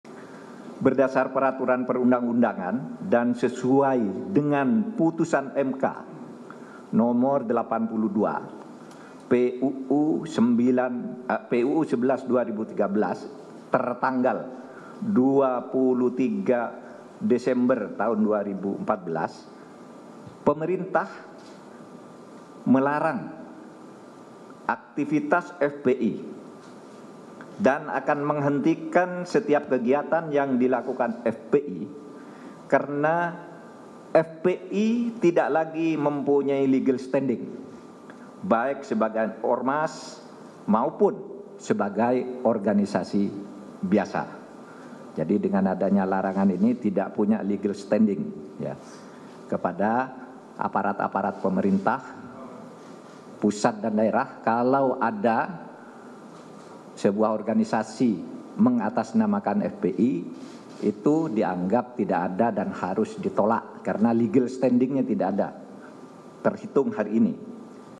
Menko Polhukam: FPI Tidak Lagi Punya Legal Standing
JAKARTA, KOMPAS.TV - Pemerintah pada 30 Desember 2020 siang menggelar jumpa pers di Kemenko Polhukam terkait dengan ormas Front Pembela Islam atau FPI.

Jumpa pers ini turut dihadiri oleh Mensesneg, Jaksa Agung, Menkominfo, Kapolri, Panglima TNI, Mendagri, Kepala BNPT, dan Menkumham.

Menko Polhukam Mahfud MD menyebut bahwa Front Pembela Islam atau FPI sejak tahun 2019 lalu sudah bubar. FPI secara organisasi sudah tidak ada.

Hal ini disampaikan Mahfud MD saat membuka konferensi pers di kantor Kemenko Polhukam.